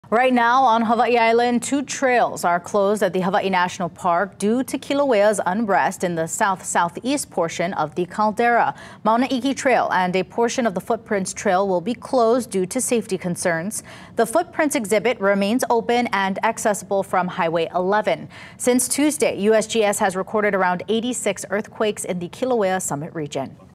Hawaii National Park closes two trails amid Kilauea's southern caldera turmoil
Due to episodic unrest in the area south-southwest of Kīlauea caldera, several trail closures are in effect in Hawaiʻi Volcanoes National Park.